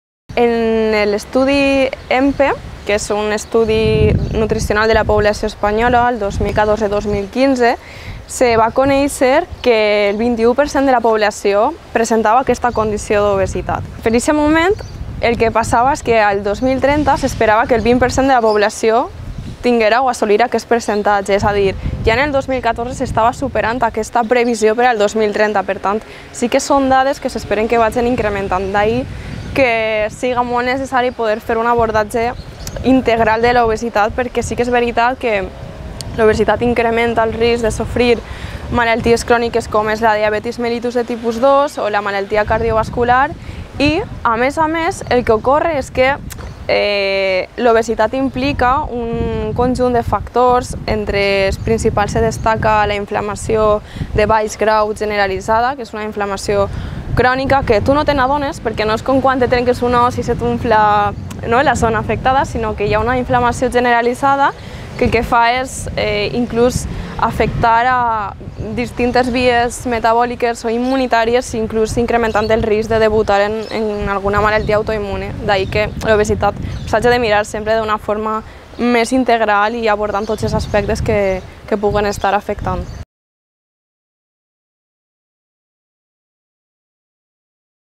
L'obesitat, un dels principals objectius a batre dels i les nutricionistes
En l'estudi ENPE, el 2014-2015, es va conèixer que el 21% de la població presentava aquesta condició d'obesitat, quan s'havia fixat assolir aquest percentatge el 2030. "Són dades que s'espera que es vagin incrementant i, per això, considerem molt important fer un abordatge integral de l'obesitat, per prevenir així malalties com la diabetis mellitus de tipus 1, problemes cardiovasculars, la hipertensió arterial, problemes d'artrosi i alguns càncers. A més, implica la inflamació de baix grau generalitzada que el que fa és afectar vies metabòliques, que pot debutar en una malaltia autoimmune". De fet, diversos estudis associen sobrepès i obesitat amb un augment de la mortalitat.